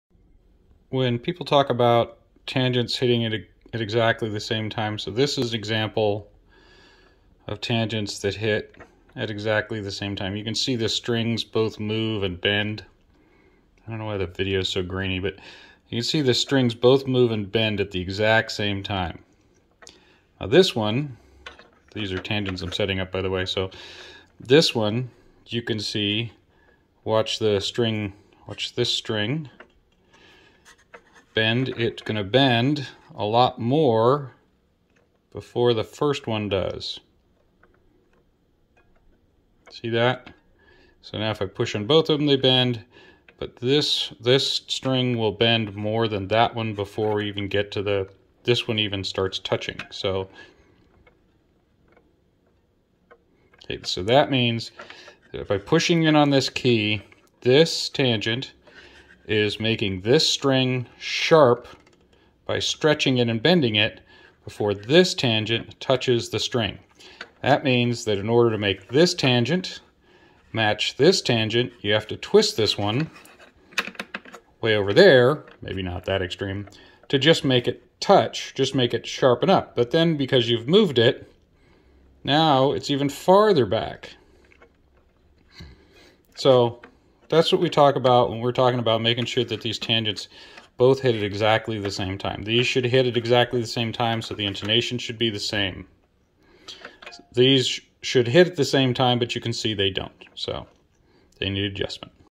Why Tangent Alignment is Important
quick video showing why tangent alignment is important for matched strings.